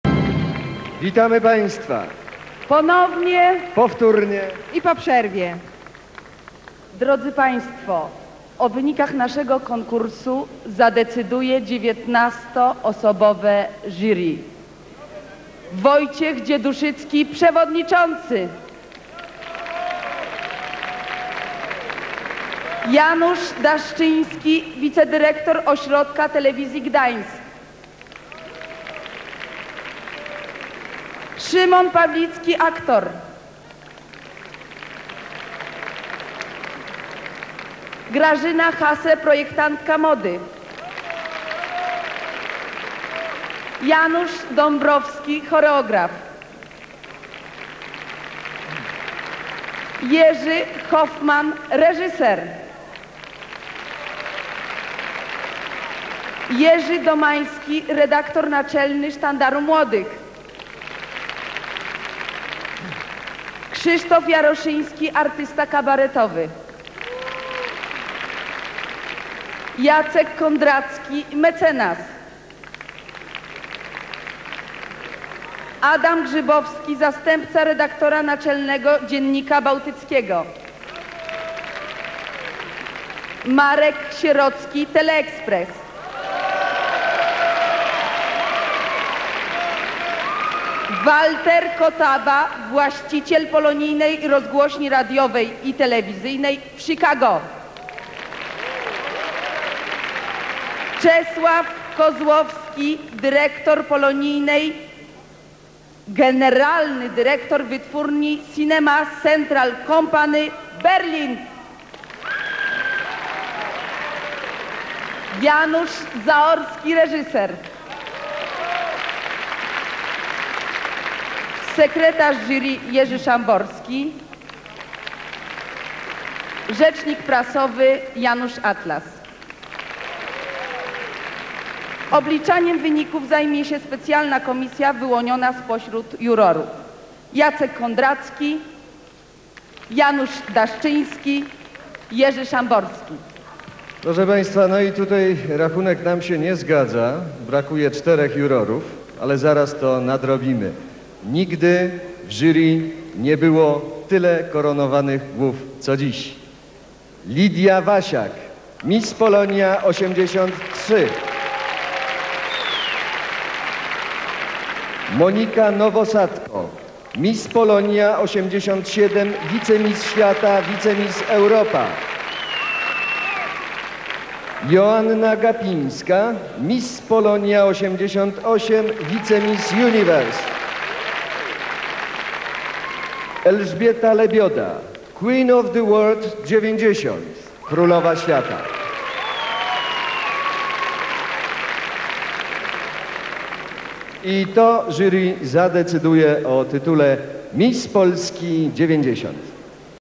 Miss Polska 1990. Jury.
Miss Polska 1990. Opera leśna w Sopocie. 6 lipca 1990 roku. Prezentacja Jury...